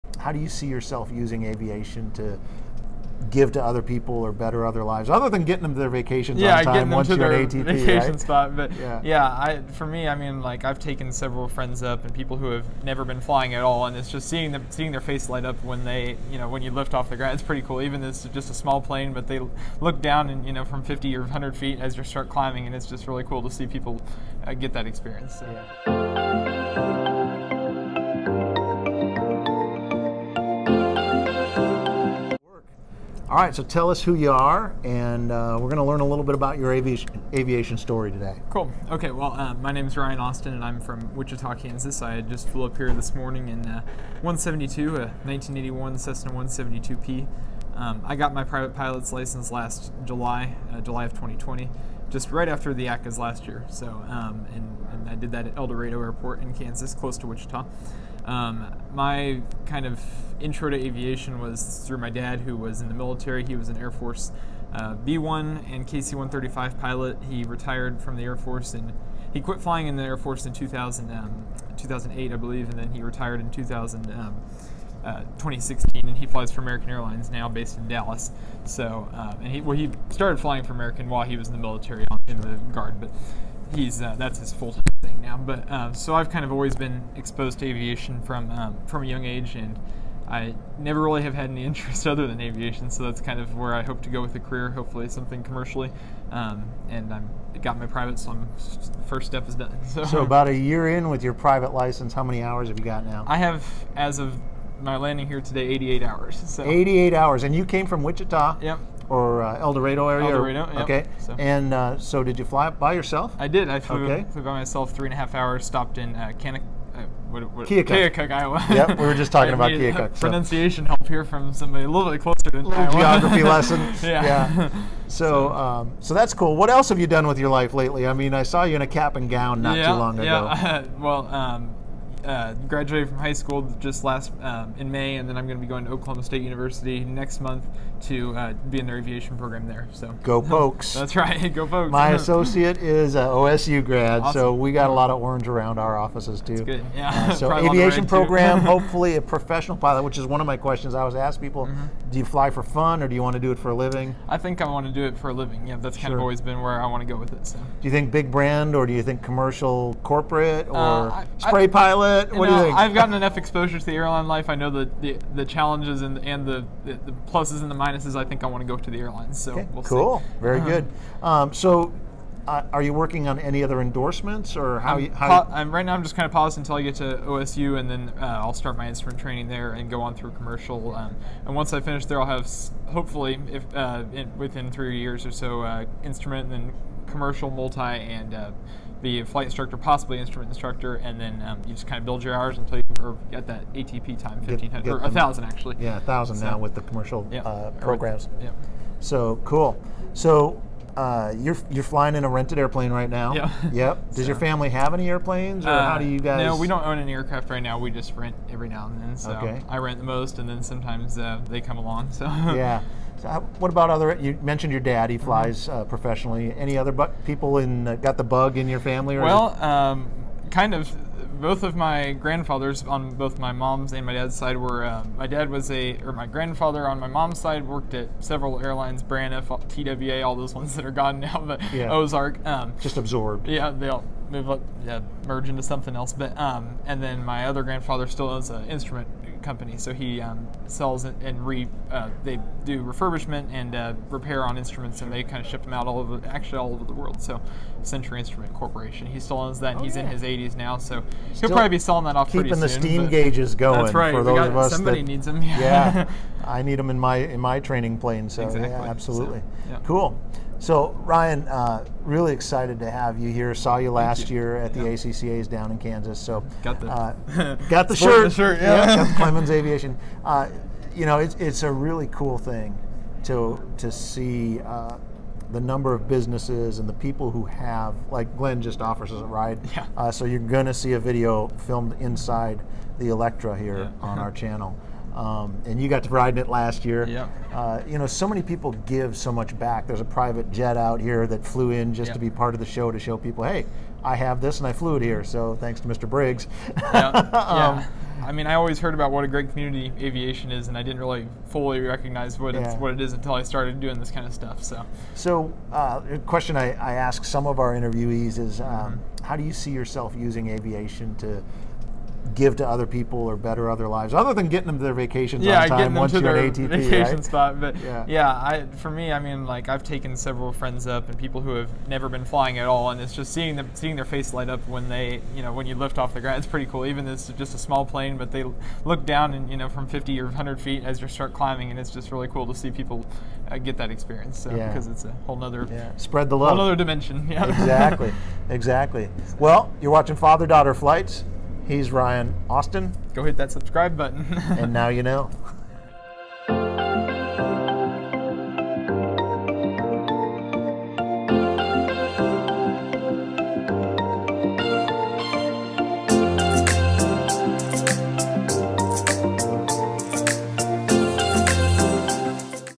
Now You Know - Ryan Austin
Another friend we have met through aviation in the past two years.  In the past 12 months, Ryan has earned his PPL, graduated from High School, and set off on the adventure of a career in commercial aviation.

Need some gauges? Check out centuryinstruments.net. Tell Grandpa Paul that Ryan sent ya!  :-)